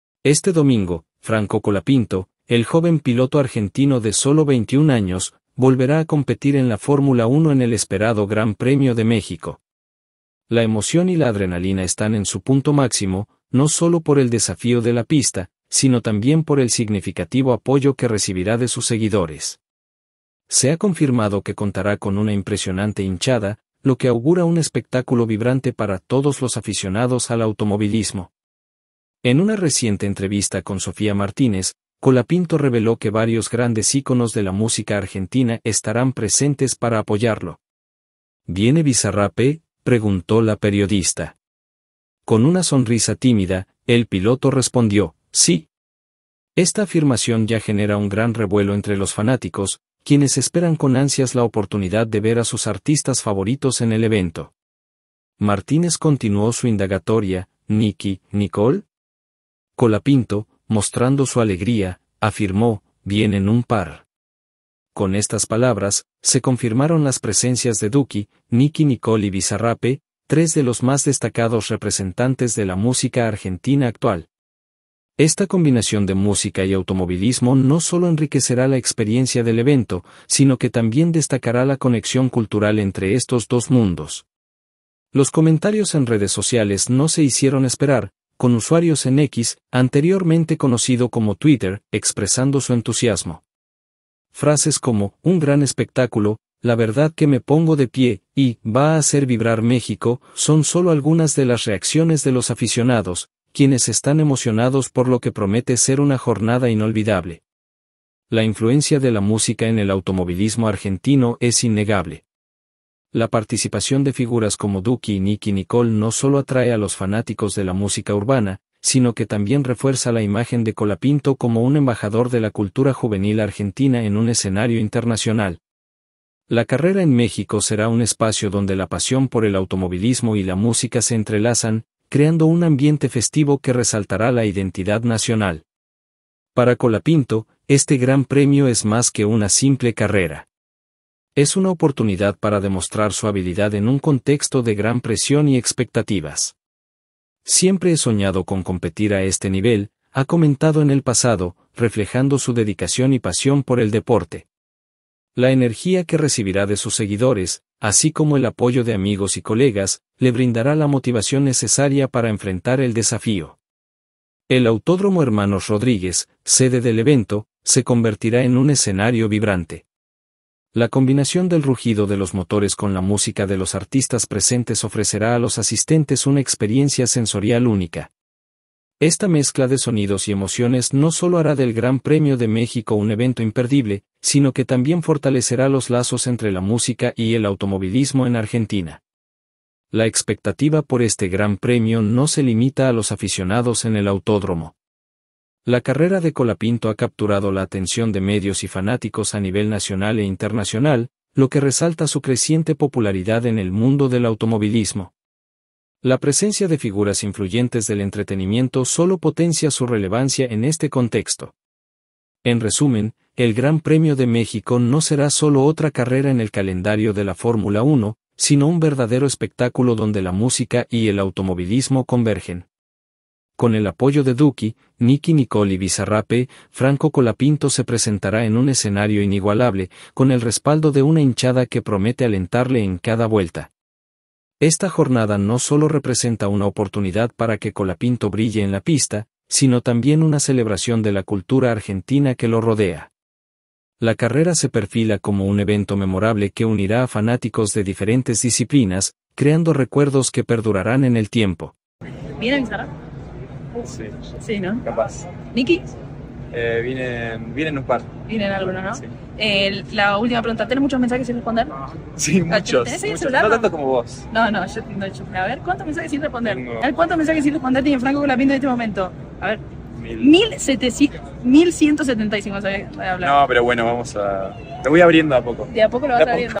FRANCO COLAPINTO TENDRÁ UNA ENORME HINCHADA EN EL GP DE MÉXICO
En este emocionante video, seguimos a Franco Colapinto mientras se prepara para el Gran Premio de México, donde contará con una impresionante hinchada.